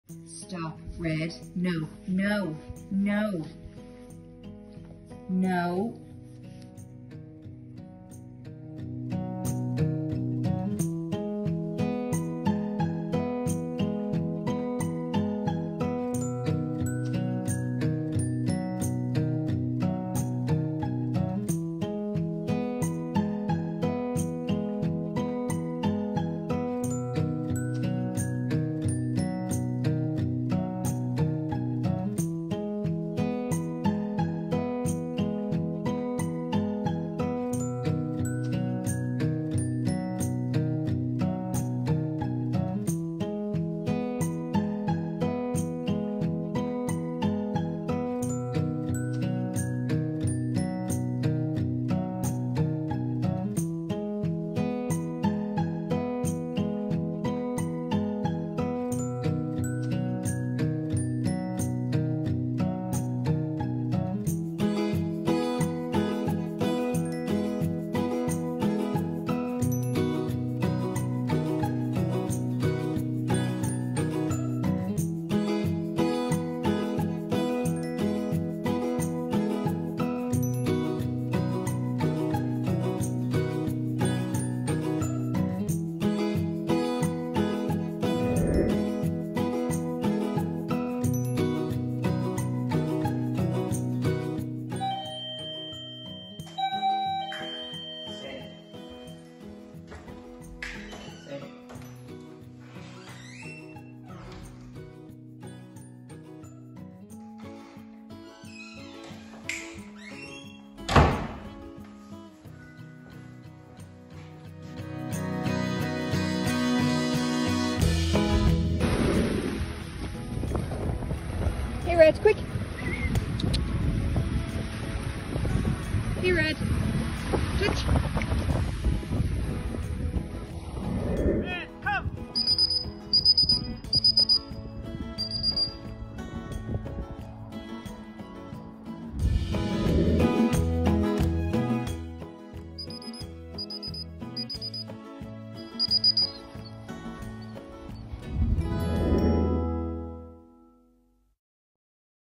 Case Study: Red The Boisterous Labrador
Red is a 13 month old fun loving yellow Labrador male, his owner was finding it very difficult to cope with his high energy and boisterous behaviour so she sought the professional help of Keith Mathews ‘The Dog Guru’ and enrolled Red on his Calmness Leads To Reward 2 Week Residential Dog Training & Behaviour Programme.  In just 2 weeks The Dog Guru was able to reverse Reds high energy and transform him into a calm, relaxed family companion.